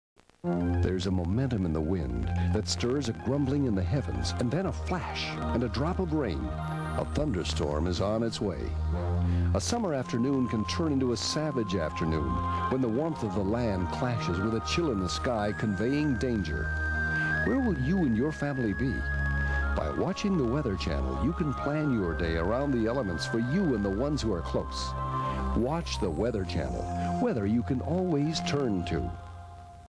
TWC Severe Weather Promo
This TWC commercial promotes the network's summer severe weather coverage (recorded in August 1995). The WeatherStar 4000 is featured in the video.